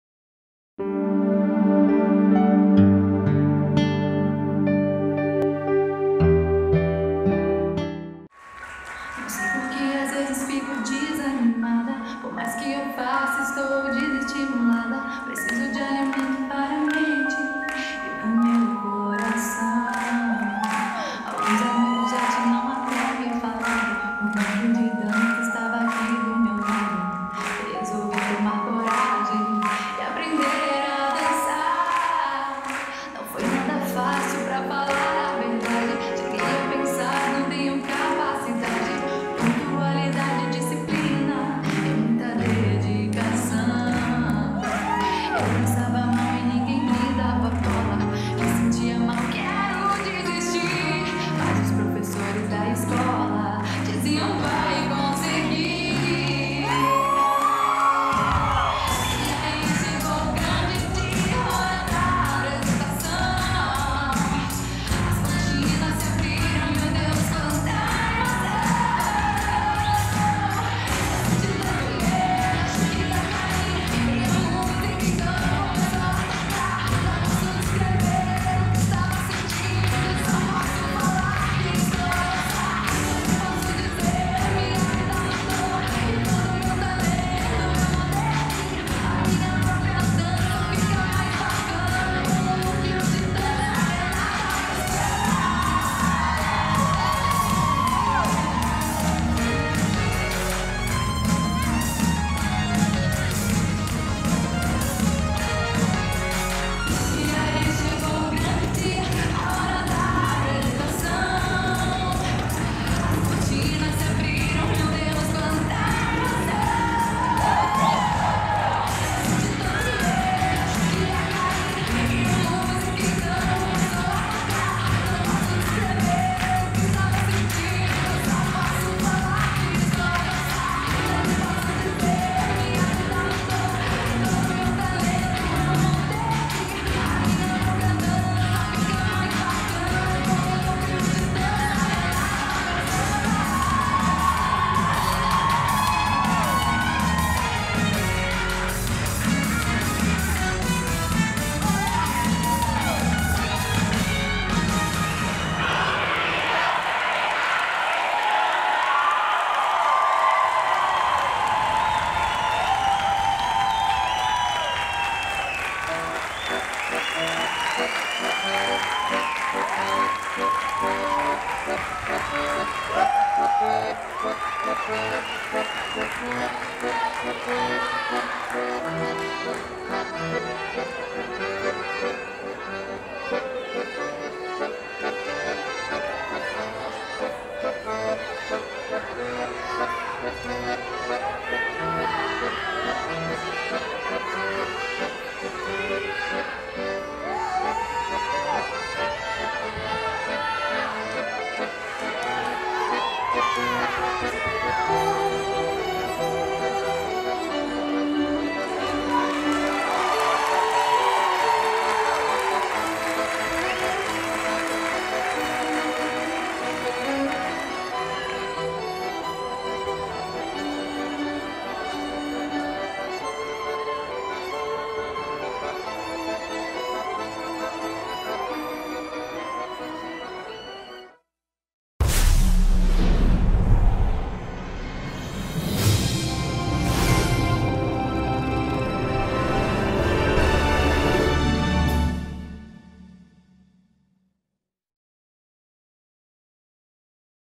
NDRP 2016 - Hino
Festival de Ballet 2016 do NÚCLEO DE DANÇA RENATA PESTANA
Teatro Munic. Araraquara - 26, 27 e 28/11/2016
Espetáculo: A Menina e o Pássaro Encantado
Coreografia: Hino